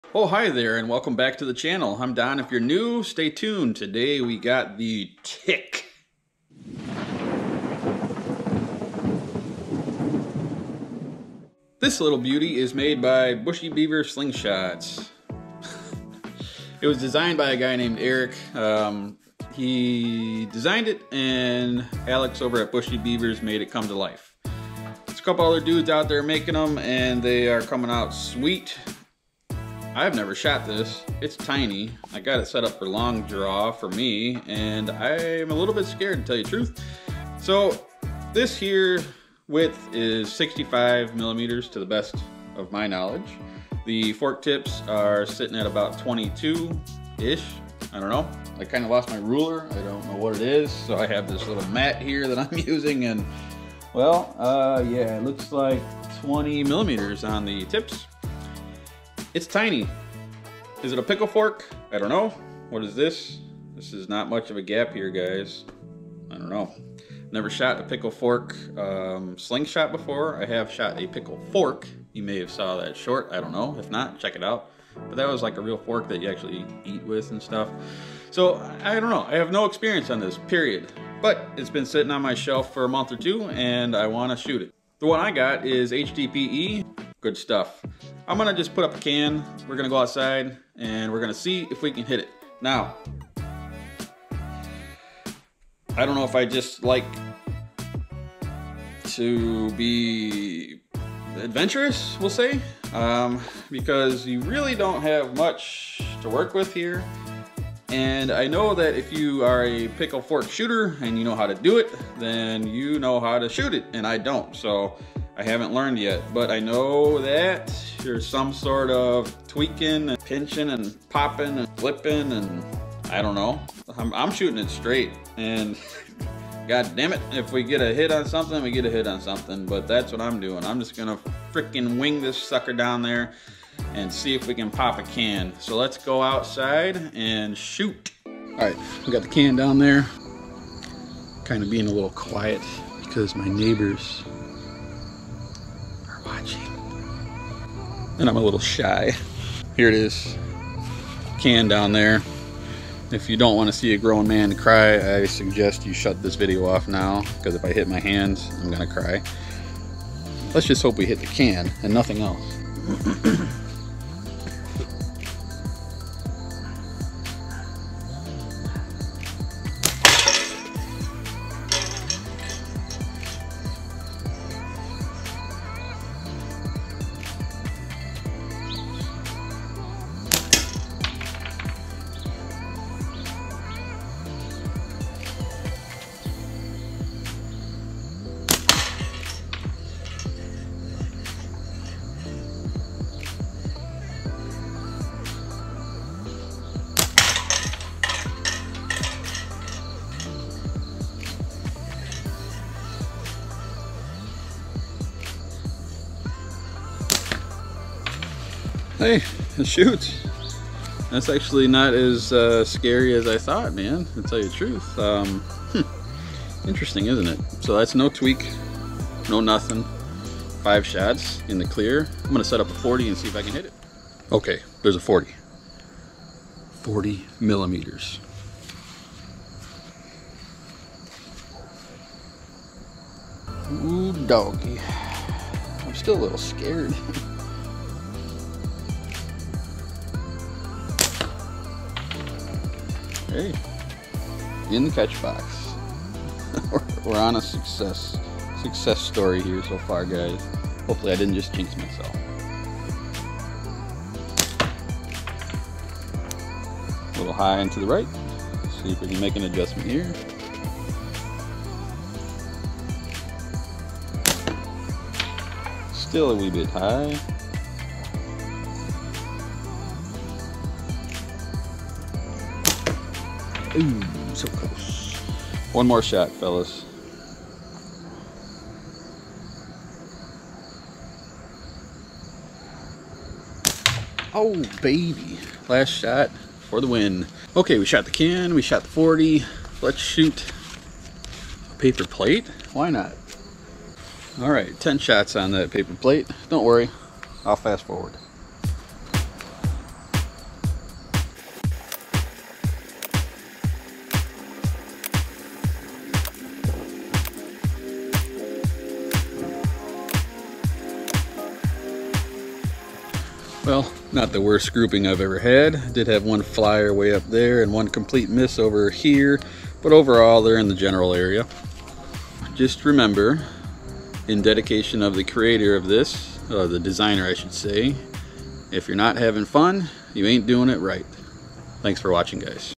PFS - The Tick - Slingshot First Shots
This is a slingshot made by Bushy Beaver Slingshots.  It’s called the Tick, it’s a small little shooter!  Very fun!  Get one here -